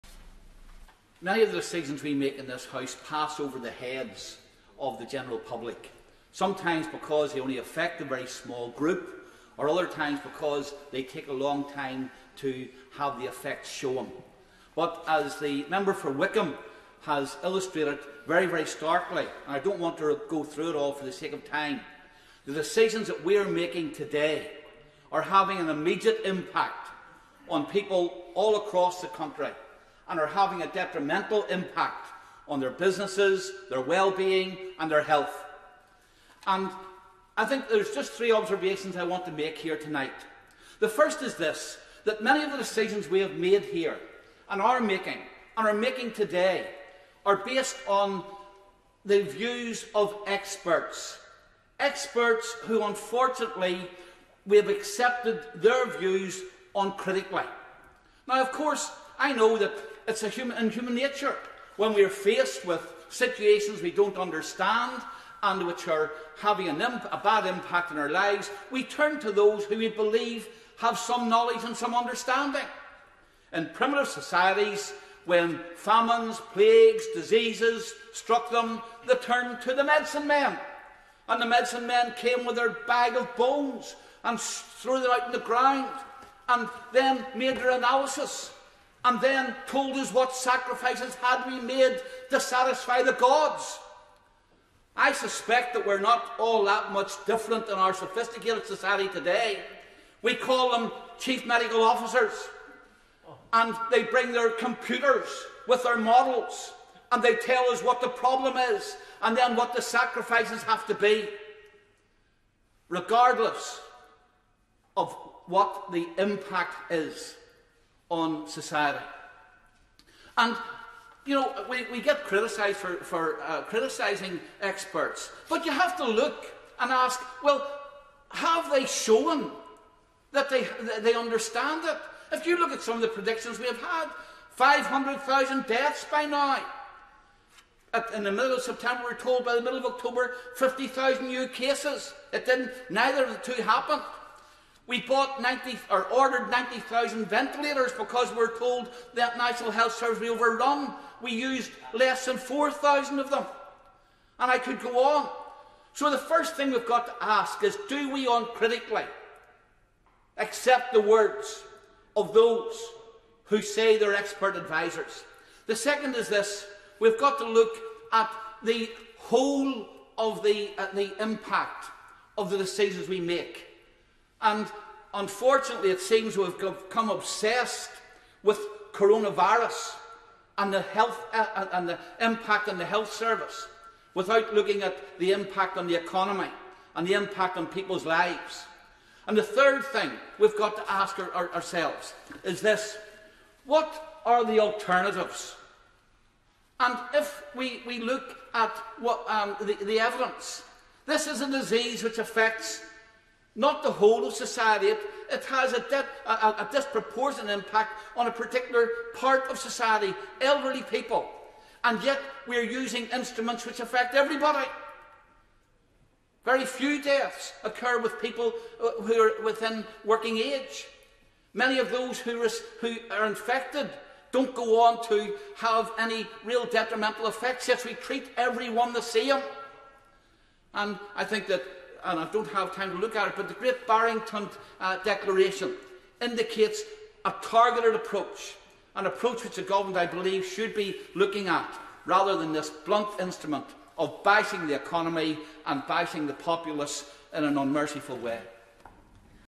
Sammy Wilson MP's Speech at Parliament's Covid-19 General Debate
Wilson speaking at Parliament's Covid-19 debate: "The decisions that we're making today, are having an immediate impact on people all across the country, and are having a detrimental impact on their businesses, their wellbeing, and their health."